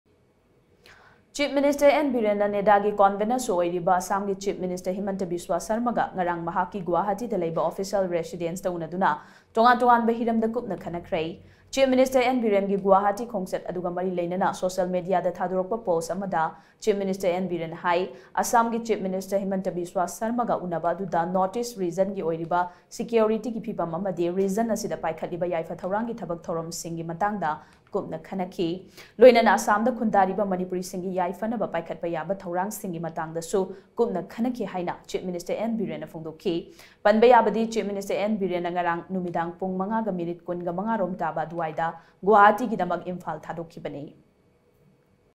CM BIREN MEETS ASSAM CM HIMANTA BISWA SARMA; DISCUSSION HELD ON DIFFERENT ISSUES
Chief Minister N Biren na NEDA gi Convenor su oiriba Assam gi Chief Minister Himanta Biswa Sarma ga ngarang mahakki Guwahati da leiba Official residence ta unnaduna tongan tonganba hiram da kupna khannakhre.